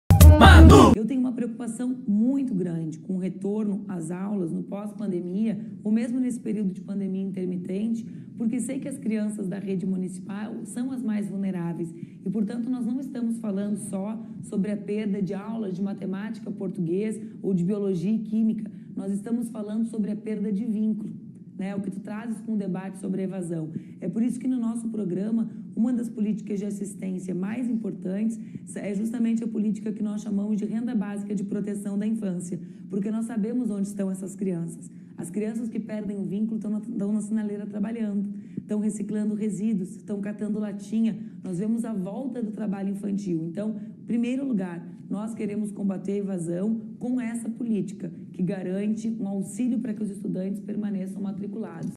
Como combater a evasão escolar? Confere nossas propostas!
O combate à evasão escolar é um dos nossos principais compromissos, por isso elaboramos o plano Renda Básica de Proteção da Infância. Por uma Porto Alegre que cuide das crianças, vai de 65!